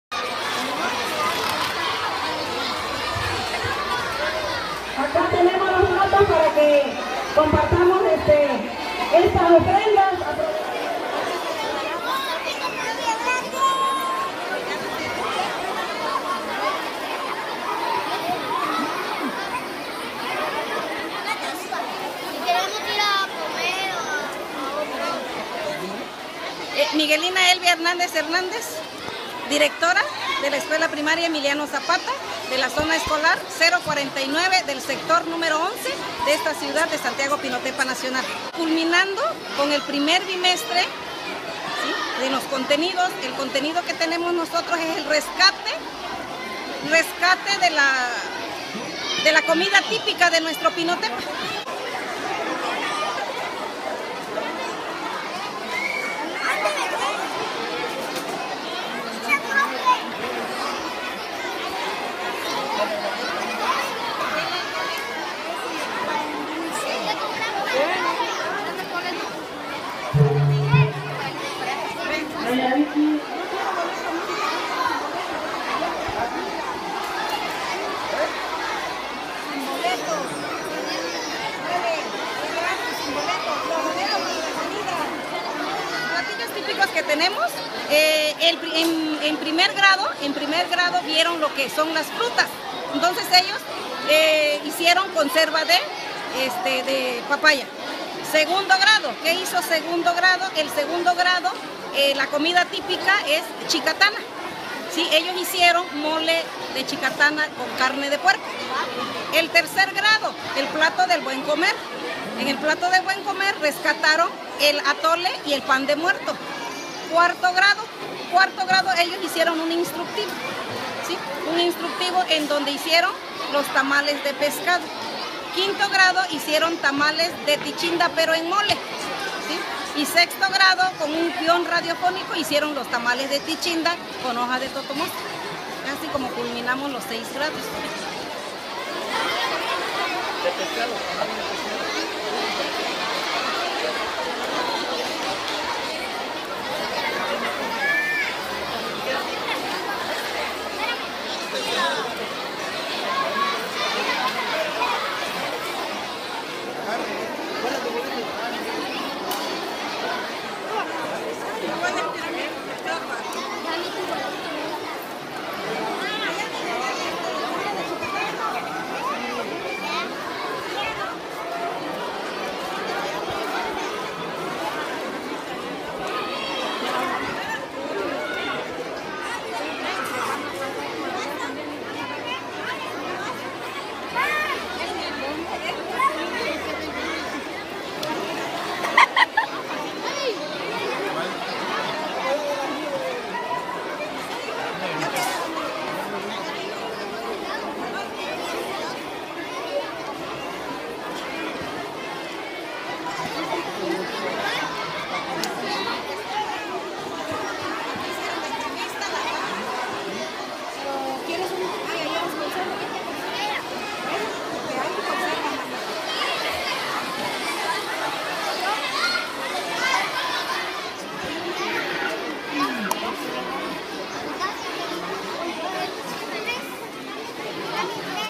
Exposición de comidas típicas de Pinotepa Nacional
La escuela primaria Emiliano Zapata realizó una exposición de comidas típicas de Pinotepa